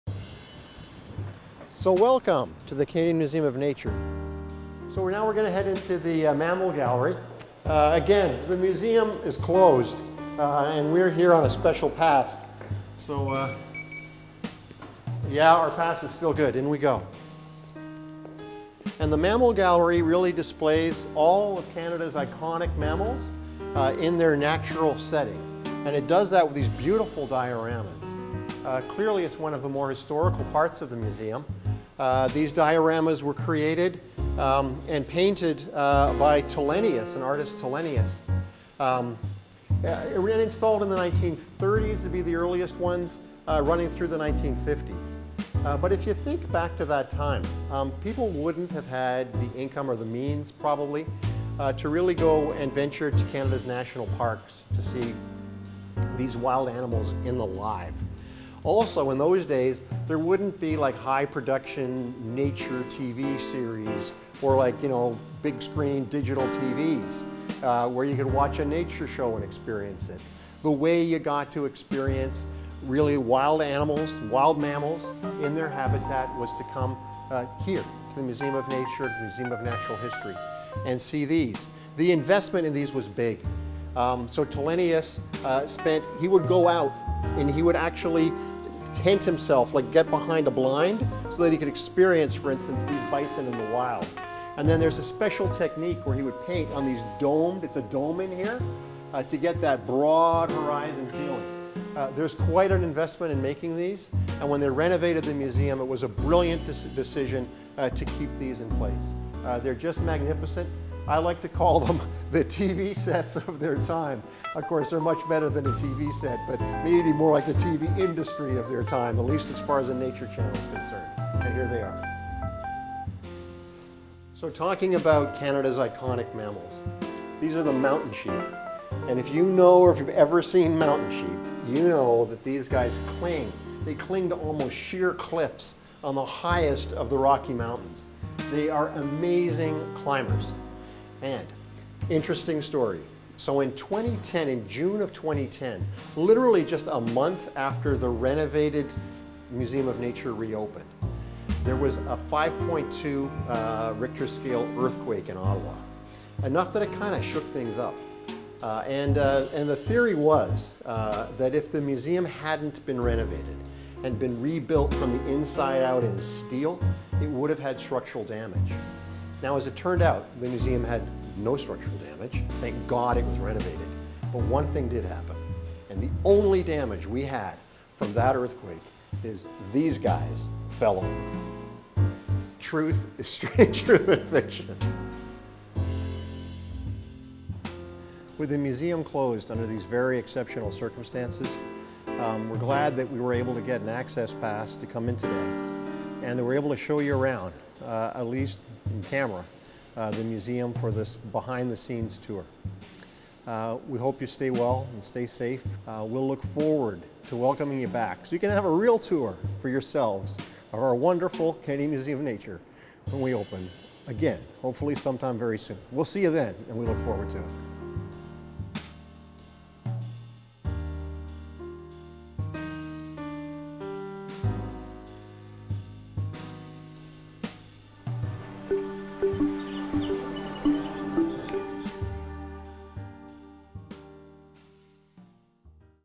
See Canada’s iconic wildlife in this special virtual tour
Get “up close” to Canada’s iconic wildlife in this guided, virtual tour of the Museum’s Mammal Gallery.

See the beautiful, historic dioramas, many of which were painted by renowned Canadian wildlife artist, Clarence Tillenius.